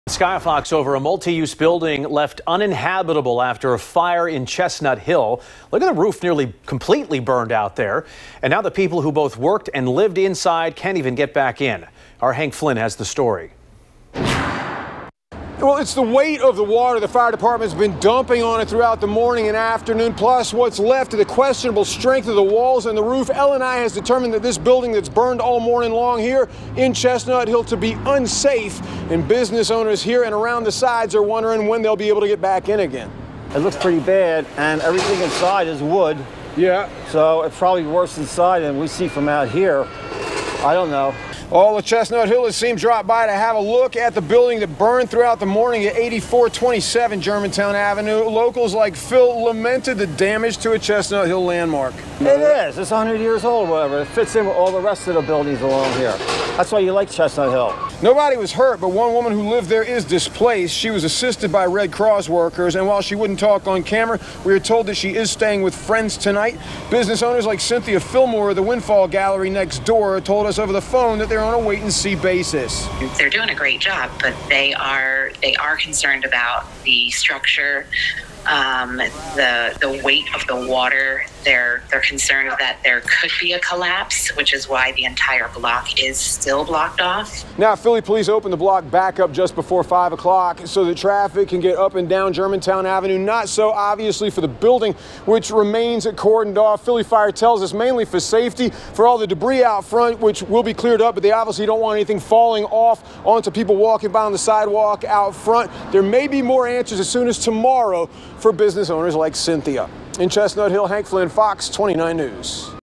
Two-alarm fire torches building in Philadelphia, no injuries reported
A two-alarm fire tore through a Chestnut Hill multipurpose building early Tuesday morning, leaving at least one woman displaced. While no injuries were reported, business owners are left wondering when they'll be able to reopen after Philadelphia's Department of License and Inspection deemed the building unsafe.